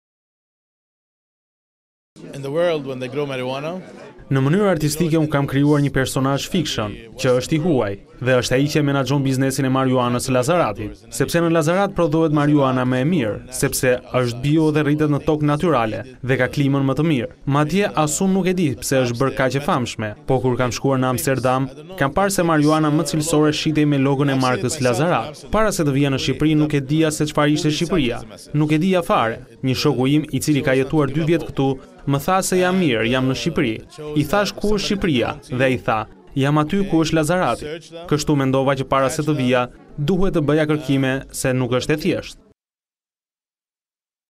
Report TV - Producenti hollivudian: Marijuana e Lazaratit më e kërkuara në Holandë
Issa Zaroui, producenti ekzekutiv i filmit hollivudian “Lazarat Burning” ka rrëfyer motivet që e kanë nxitur të realizojë këtë film për çështjen e shumëpërfolur të marijuanës që u mboll në Lazarat. Ndërsa në Amsterdam Zaroui ka parë që të shitej legalisht marijuana bio e “markës” Lazarat, vete Lazarati dhe Shqipëria në atë kohë digjej nga problemet e pasojat e kultivimit te saj ilegal.

Vetëm ky motiv, ky kontrast që ka vënë re Zaroui të bën të shpresosh se në vjeshtën e vitit 2017 producentët e aktorët e “Mercury Films”, ku drejtor është një nga emrat më të njohur të Hollivudit, Uilliam Kaufmann, do të sjellin një film me vlera. Në këtë film personazhi kryesor është një shqiptar që quhet Bujar dhe ky do të luhet nga aktori i famshëm hollivudian Gabriel Garko.